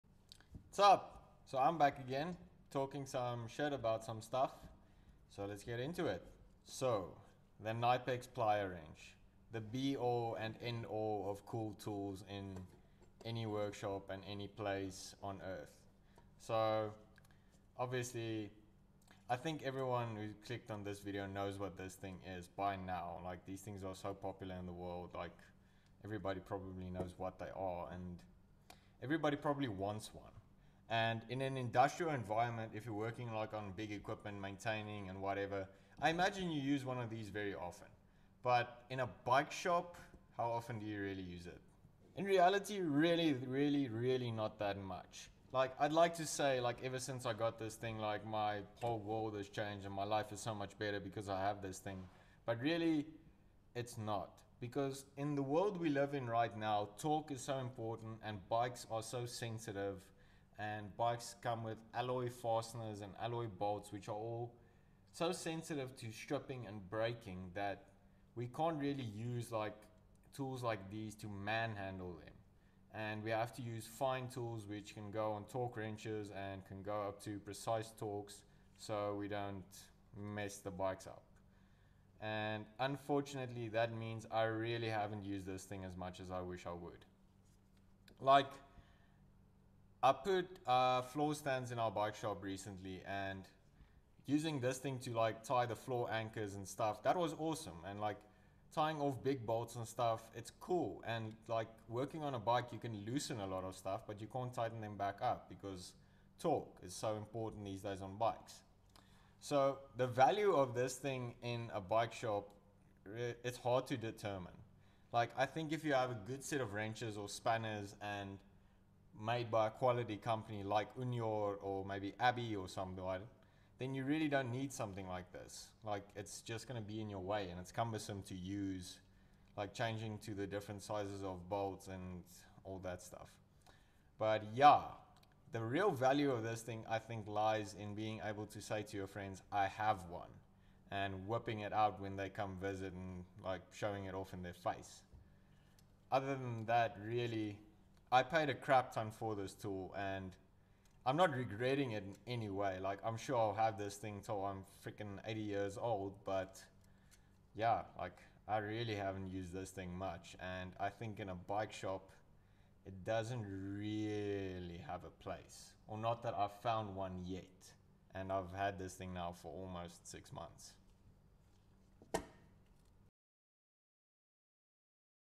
Is the KNIPEX PLIER WRENCH worth it ? | 2023
Just my quick summary of what I think of my plier wrench A couple of months into getting it.